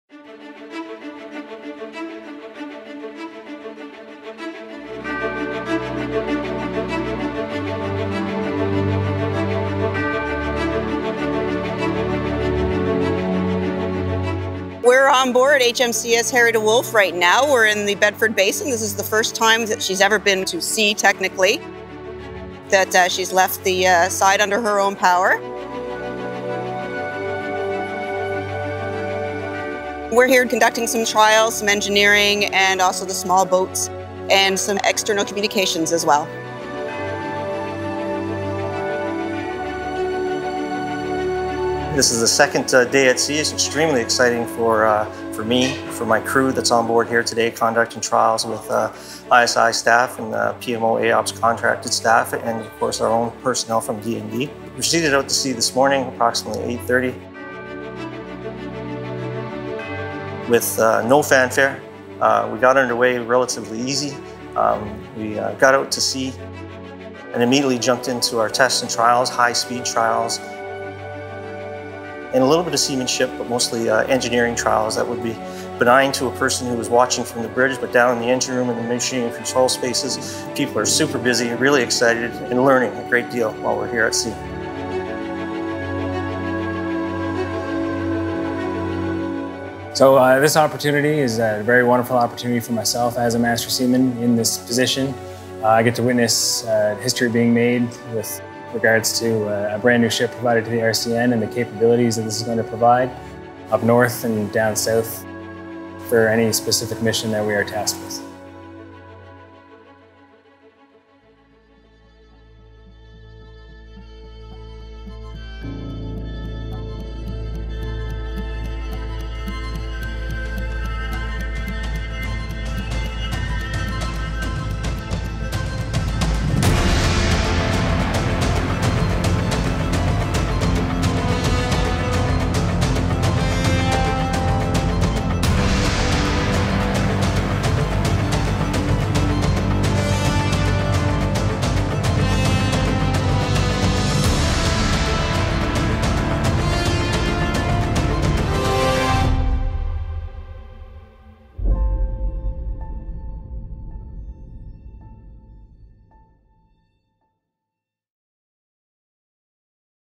To SEA! - Building Harry DeWolf Part 5/7
The Arctic and Offshore Patrol Ship (AOPS) project will deliver six ice-capable ships, designated as the Harry DeWolf class, after Canadian wartime naval hero Vice-Admiral Harry DeWolf. The official RCN ship's class designation will be Arctic and Offshore Patrol Vessel (AOPV).

The AOPV will be capable of:

- Armed sea-borne surveillance of Canada's waters, including the Arctic
- Providing government situational awareness of activities and events in these regions
- Cooperating with other partners in the Canadian Armed Forces and other government departments to assert and enforce Canadian sovereignty, when and where necessary
conducting a diverse range of missions, worldwide.

Construction of the first AOPV began in 2015, with HMCS Harry DeWolf scheduled for delivery in 2019.